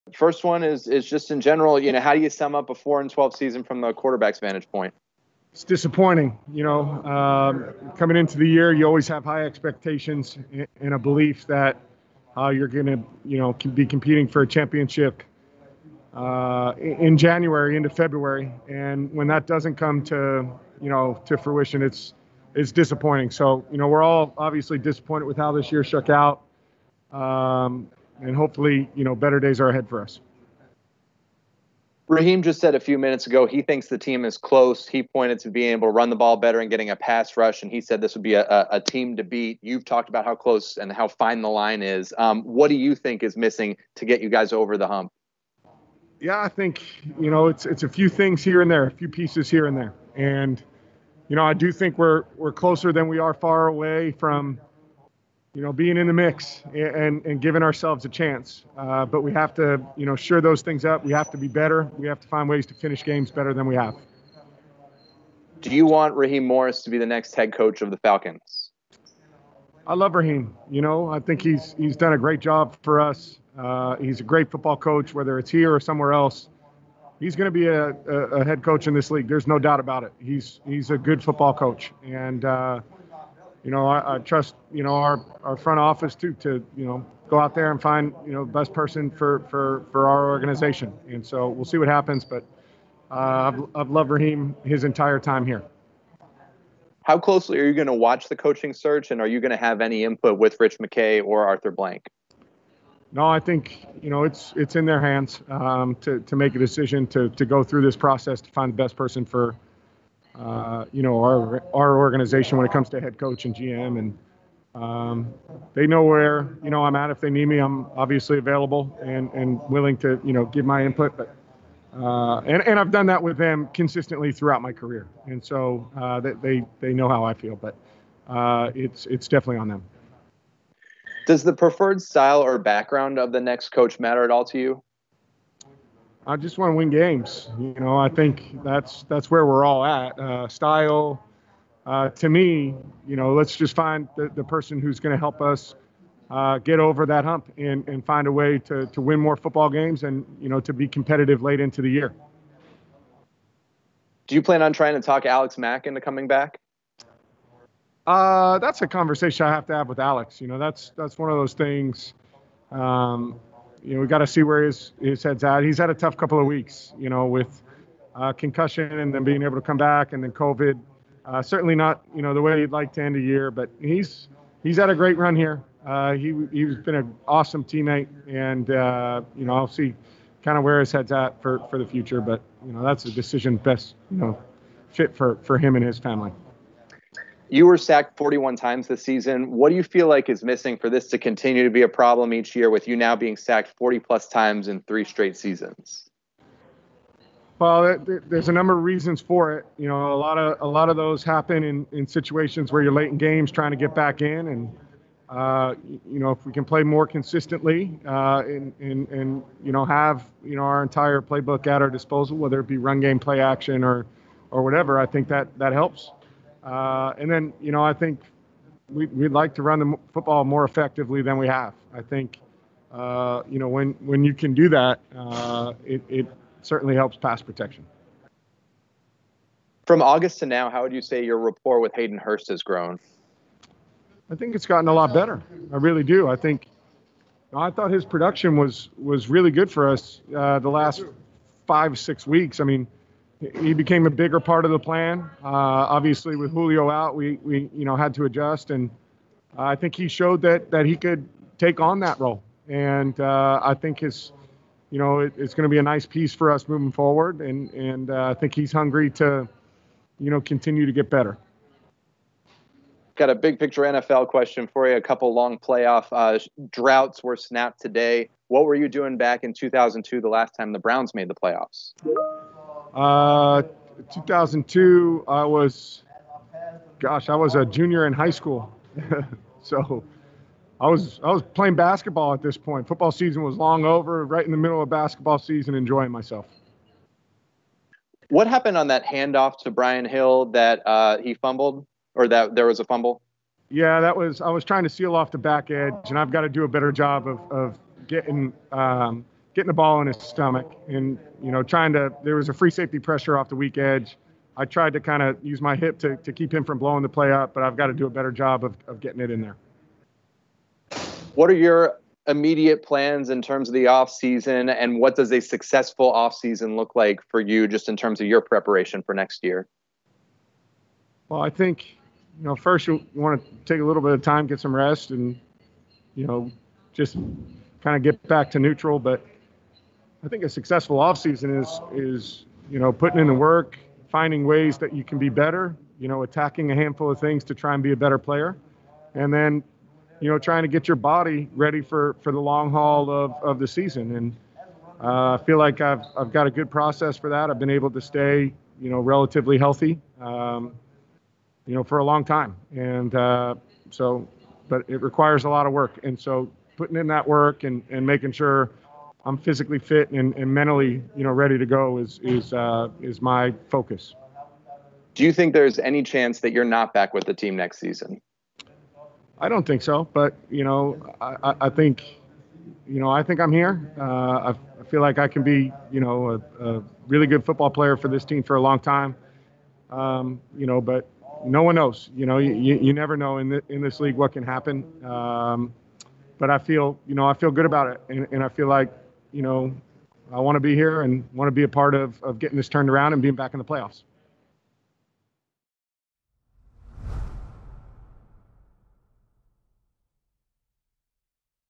Matt Ryan: Postgame Press Conference | Week 17 vs. Tampa Bay Buccaneers
Matt Ryan talks to the media about the week 17 loss to the Tampa Bay Buccaneers.